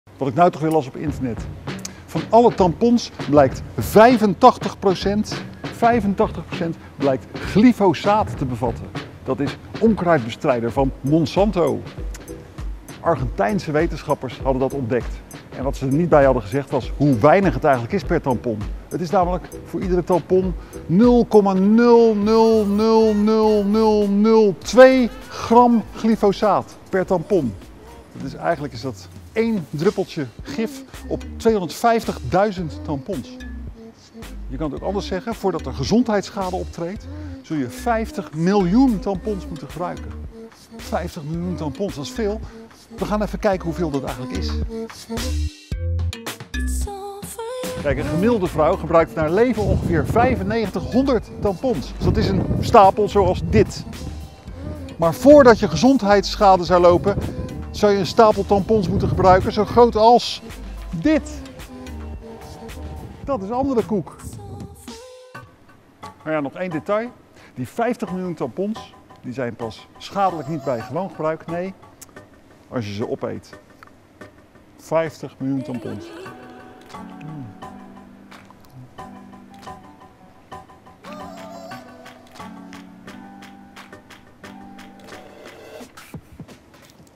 Zijn tampons kankerverwekkend door Monsanto? - de Volkskrant
85% tampons en hygiëneproducten besmet met giftige onkruidverdelger van Monsanto, staat te lezen op verschillende websites. Maar klopt dat wel? Wetenschapsredacteur Maarten Keulemans legt het uit.